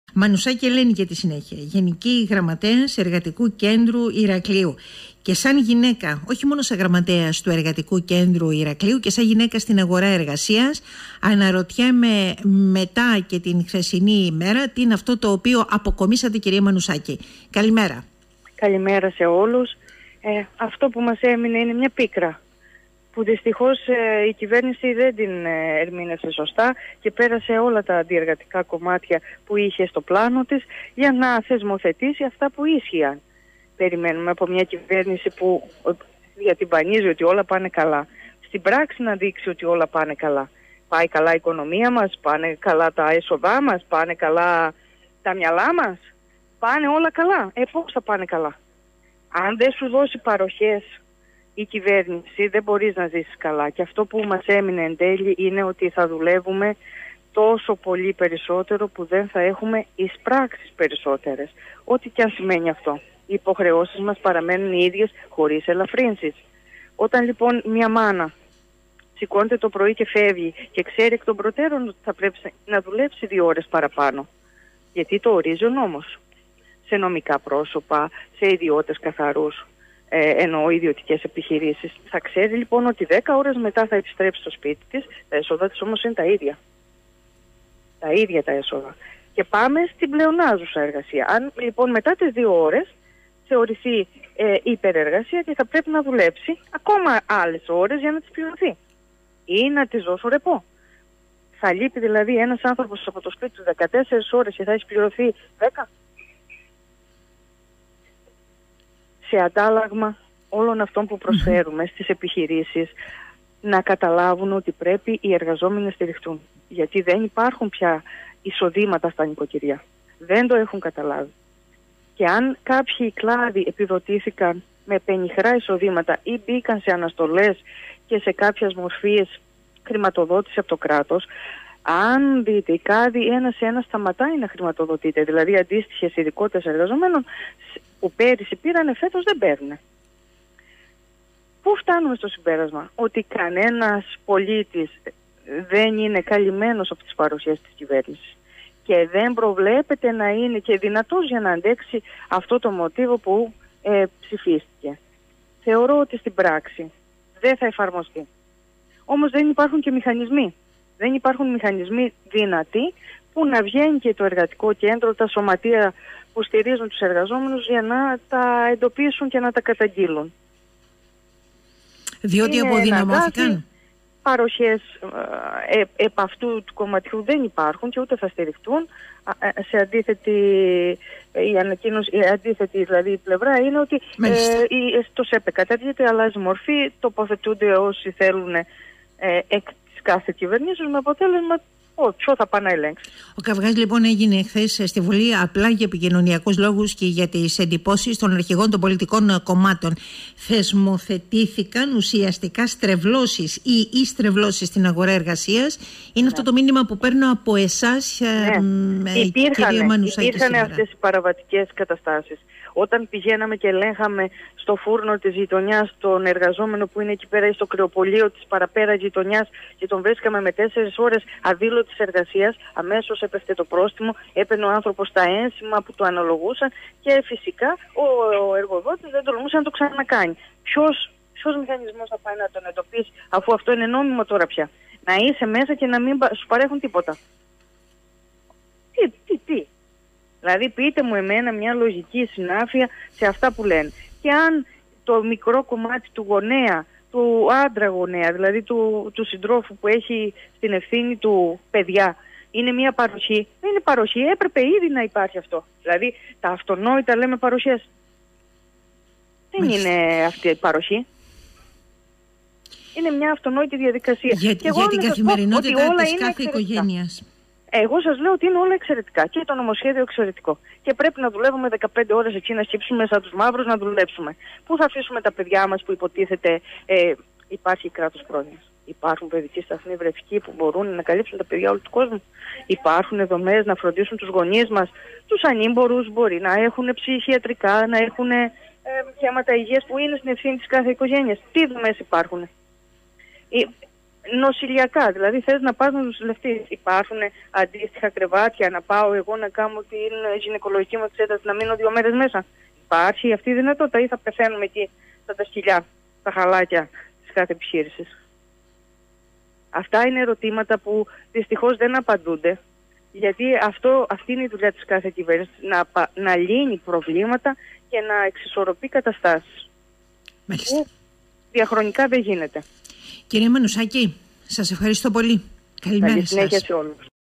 Ε. Μανουσάκη: Πίκρα σήμερα για τους εργαζόμενους
«Μεγάλο το πλήγμα στον οικογενειακό προϋπολογισμό και τις εργαζόμενες γυναίκες»
Στη νομιμοποίηση των στρεβλώσεων που επικρατούν στην αγορά εργασίας σε πραγματικό χρόνο, τις εργαζόμενες γυναίκες, τους εργαζόμενους συζύγους, τις υποχρεώσεις τους παράλληλα με την ανατροφή των παιδιών τους αναφέρεται η ΓΓ του ΕΚΗ Ελένη Μανουσάκη μιλώντας στον 98.4.